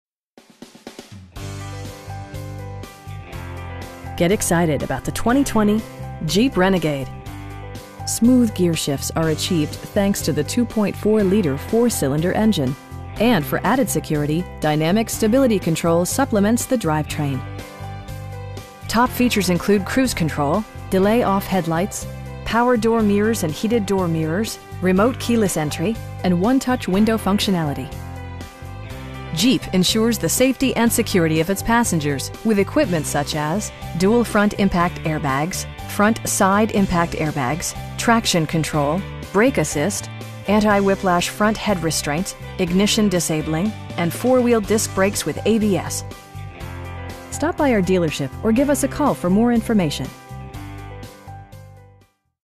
2020 Jeep Renegade SPORT FWD in Las Vegas, NV 89146-3400
Sahara Chrysler Jeep Dodge Ram

Get excited about the 2020 Jeep Renegade Smooth gearshifts are achieved thanks to the 2.4 liter 4 cylinder engine, and for added security, dynamic Stability Control supplements the drivetrain. Top features include cruise control, delay-off headlights, front bucket seats, telescoping steering wheel, power door mirrors and heated door mirrors, remote keyless entry, an overhead console, and 1-touch window functionality. Jeep ensures the safety and security of its passengers with equipment such as: dual front impact airbags, front side impact airbags, traction control, brake assist, anti-whiplash front head restraints, ignition disabling, and 4 wheel disc brakes with ABS. Our aim is to provide our customers with the best prices and service at all times. Stop by our dealership or give us a call for more information.